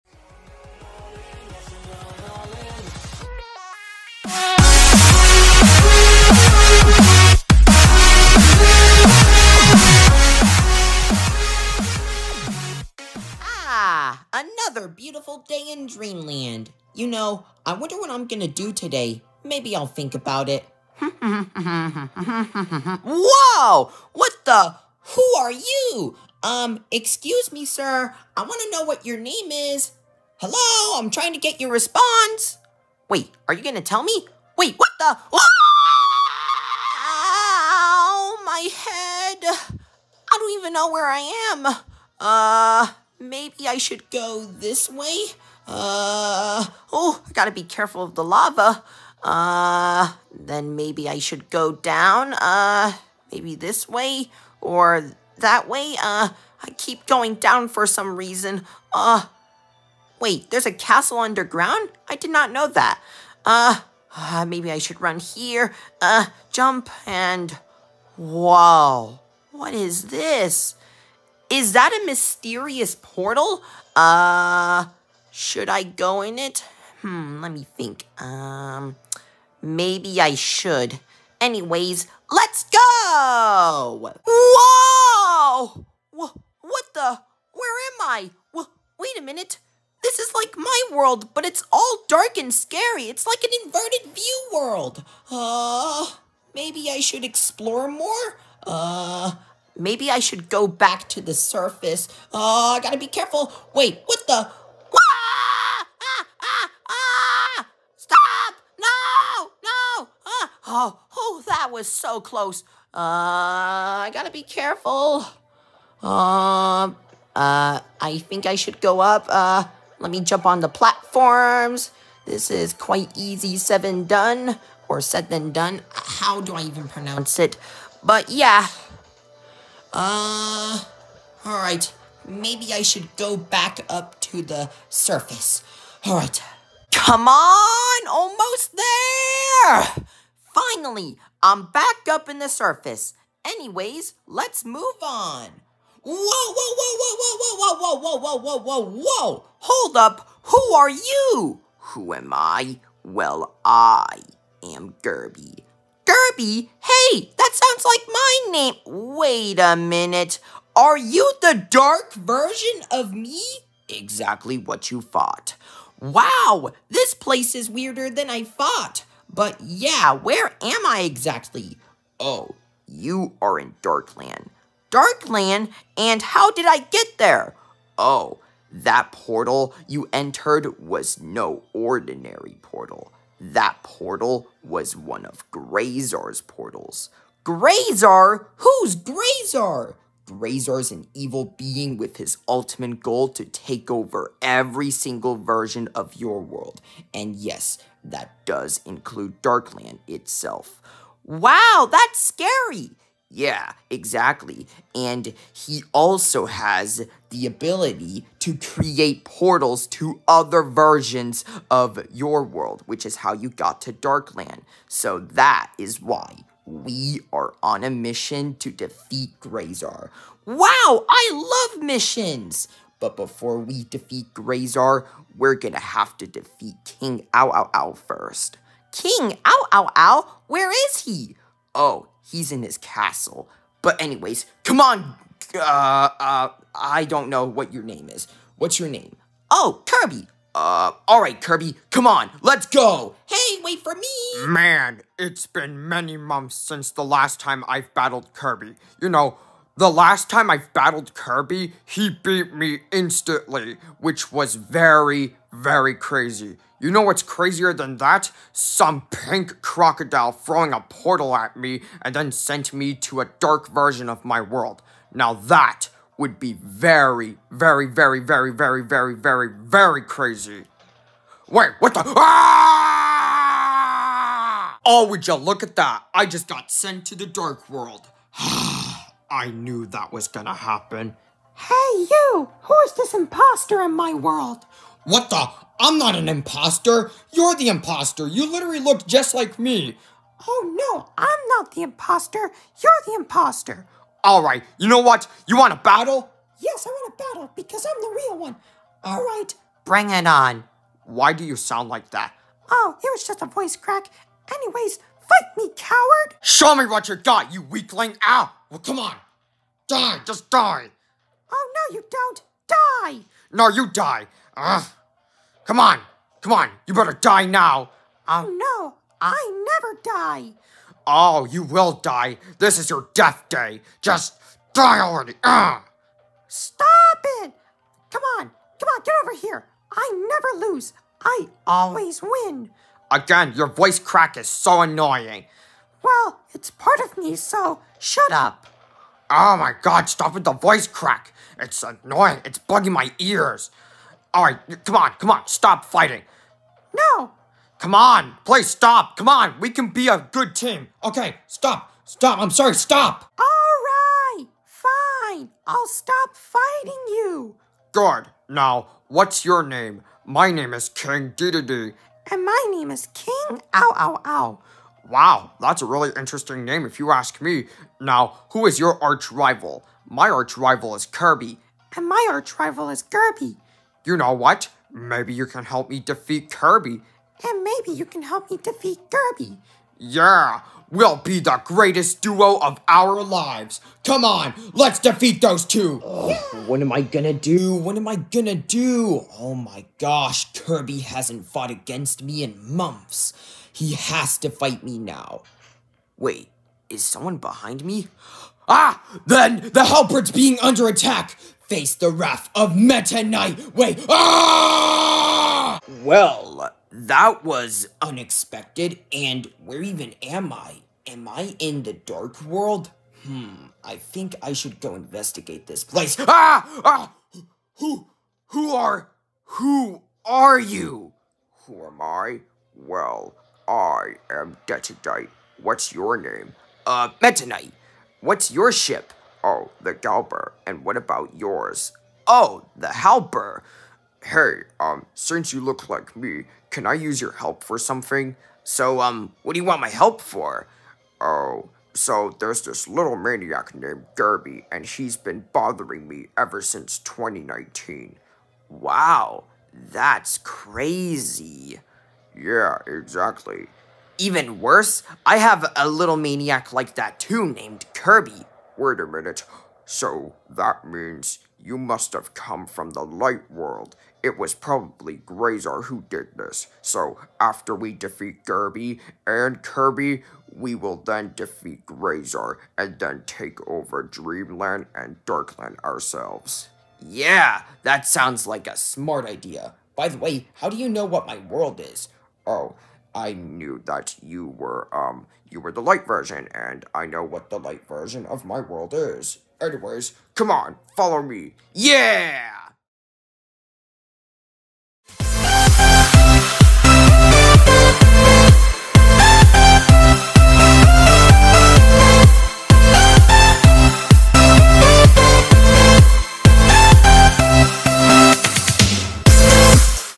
Kirby’s Dark Land - Episode 1 (From Light to Dark)
In this Video, Kirby gets thrown by a Mysterious Man and then he enters a portal to a Dark Version of his world, Then Kirby meets a dark version of himself! And the two set off for an adventure! But what awaits them here? Find out in the next episode of the series!